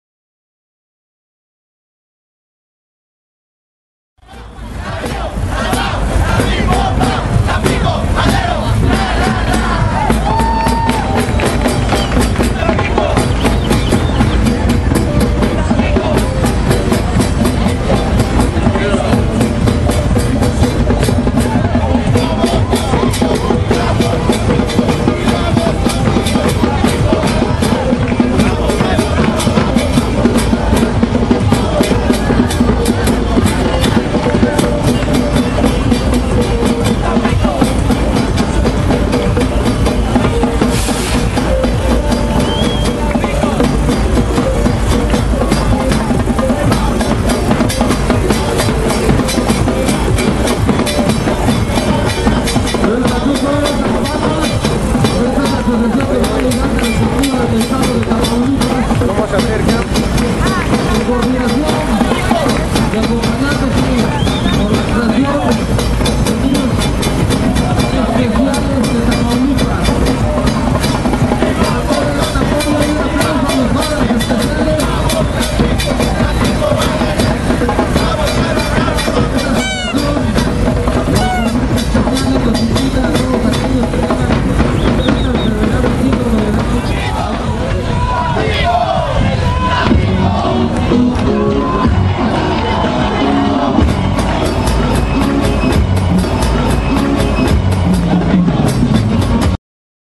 carnaval terroraizer
un pequeño video de la terro fieles seguidores de la jaiba brava